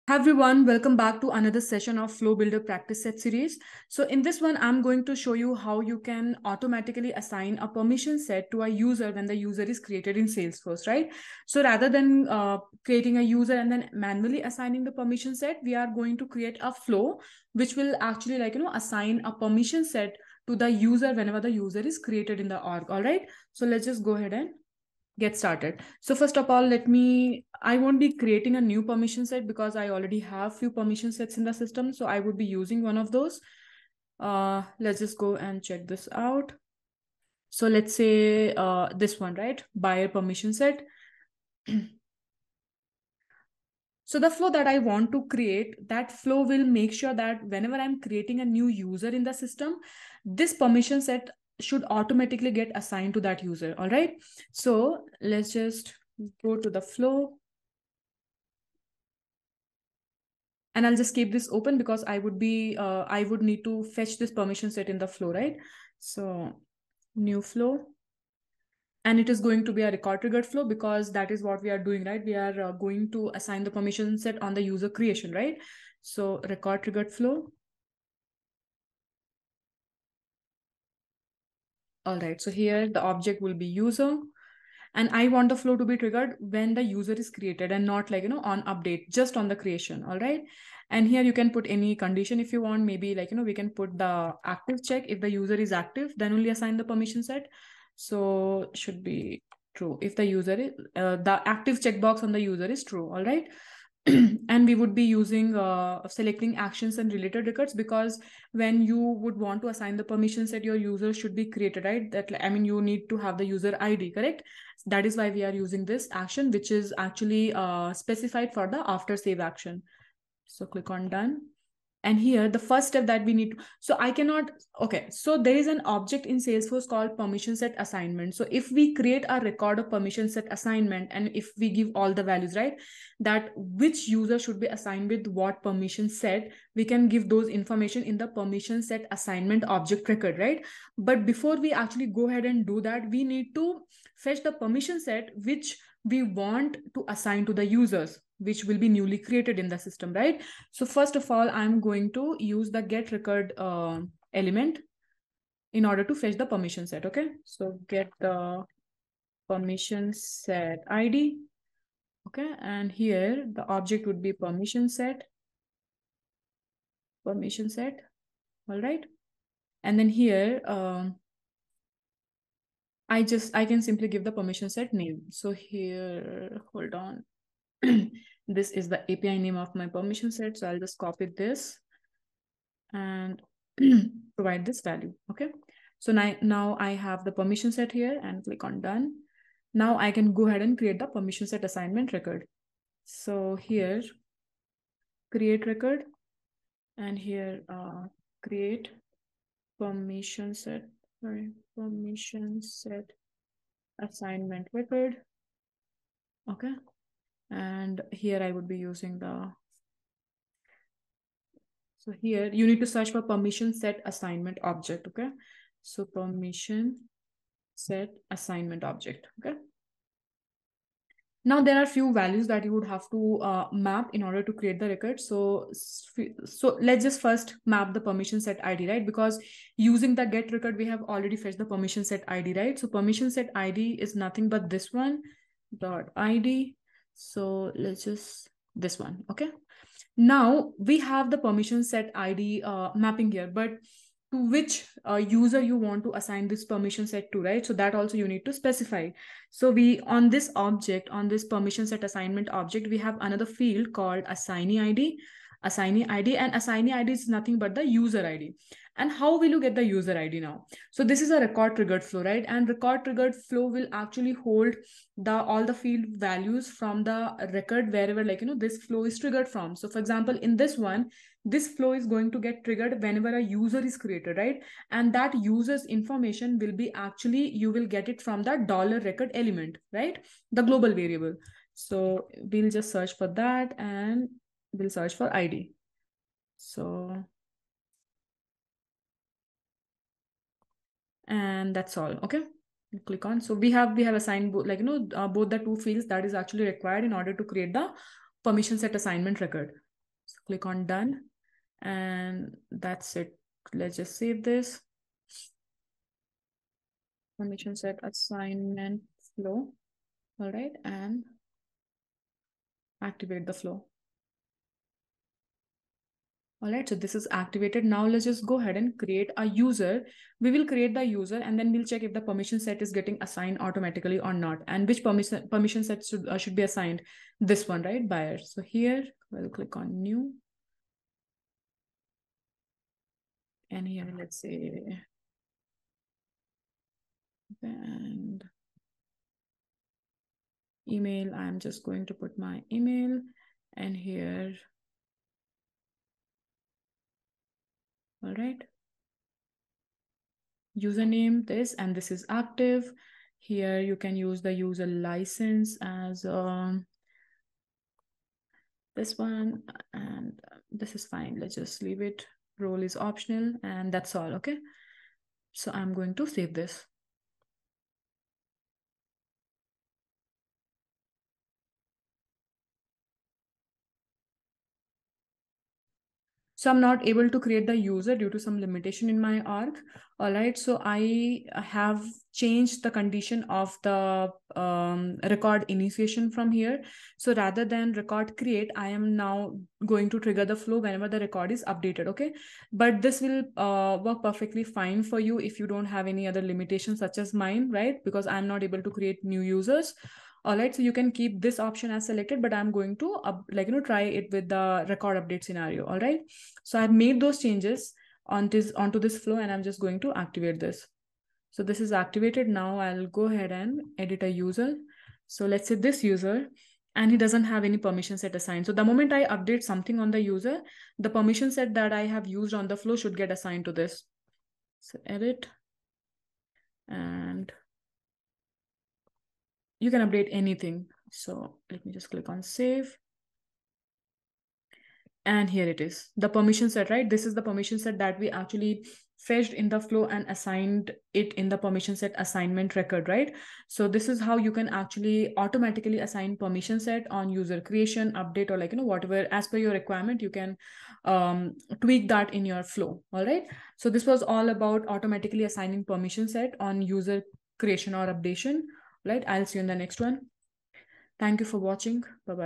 Scenario 13 : Automatic Permission Set Assignment on User Creation | Salesforce | Flow Builder
Learn how to auto assign permission set on user creation. The video demonstrates a record triggered flow which every time a user is created/updated, assigns a permission set automatically. The flow includes below elements :
- Get Permission set records in flow
- Create Permission Set Assignment Record in Flow
- Map Permission set assignment record values to the values from record triggered $Record variable while record creation.